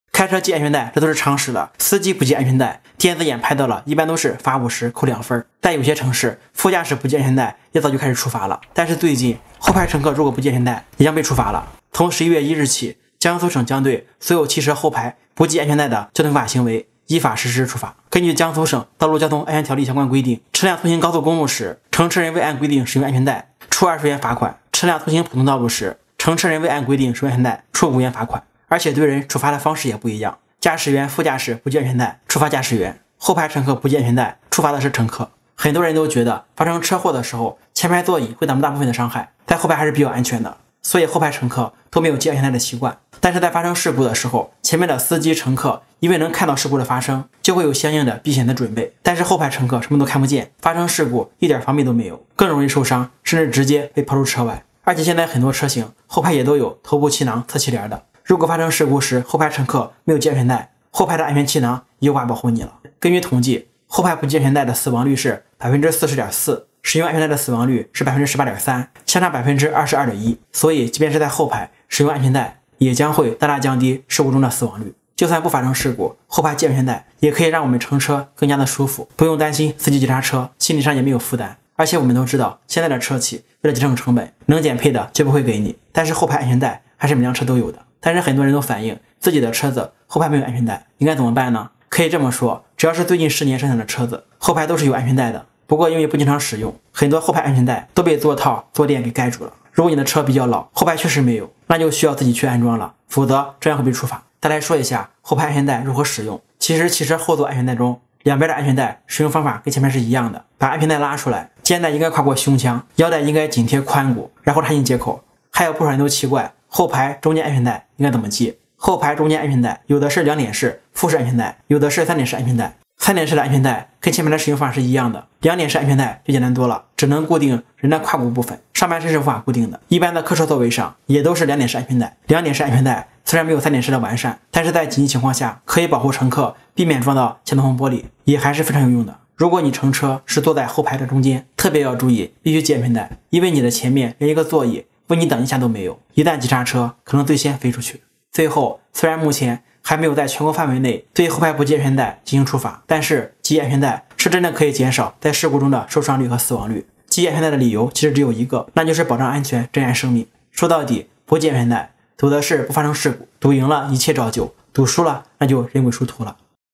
后排乘客不系安全带正式开始处罚！处罚乘客还是驾驶员？小车后排安全带应该怎么系？
开车系安全带，这都是司机的共识，但是后排的乘客系安全带，这样的应该还比较少吧，最近，后排乘客如果不系安全带，也将会被处罚了。那如果乘客不系安全带，是处罚乘客还是司机呢，汽车后排找不到安全带，没有安全带应该怎么办呢？